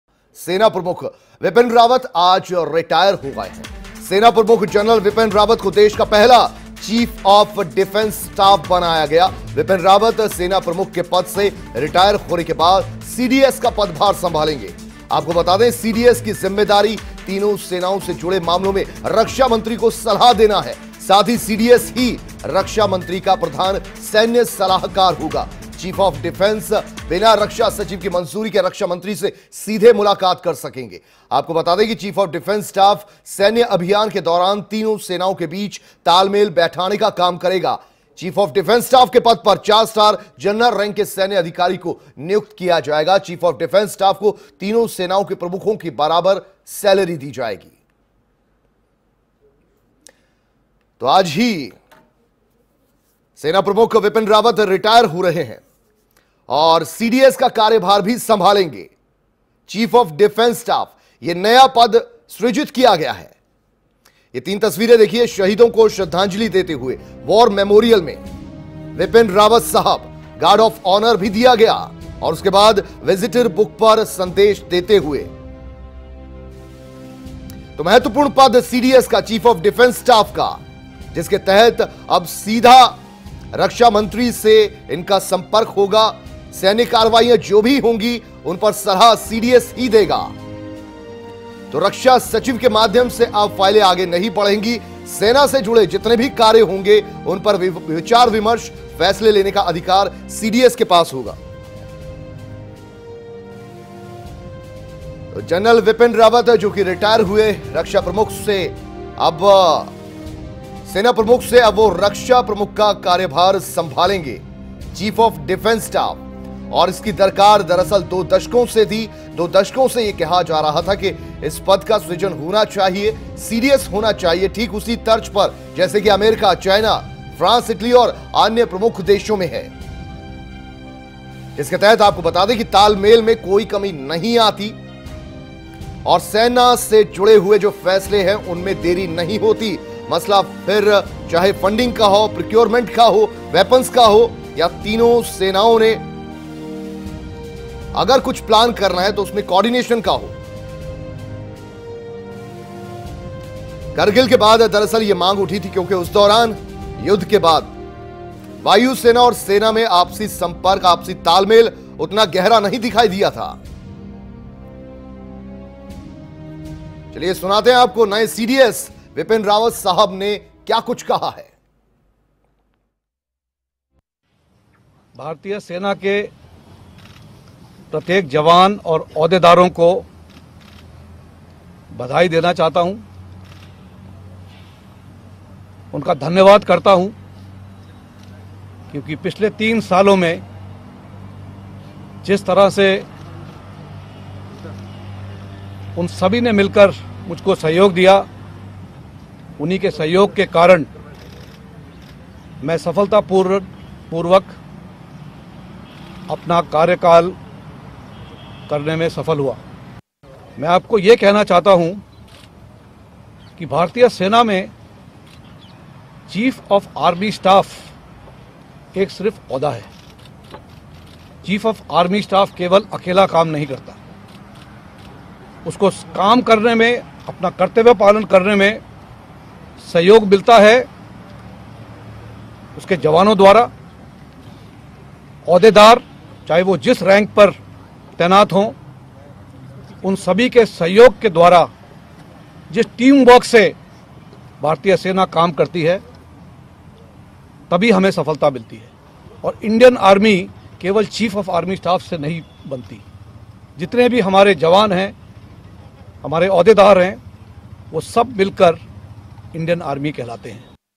BipinRawat को बनाया गया पहला CDS
सेना प्रमुख बिपिन रावत आज रिटायर हो गए हैं. सेना प्रमुख जनरल बिपिन रावत को देश का पहला चीफ ऑफ डिफेंस स्टाफ बनाया गया है…. बिपिन रावत सेना प्रमुख के पद से रिटयर होने के बाद CDS का पदभार संभालेंगे.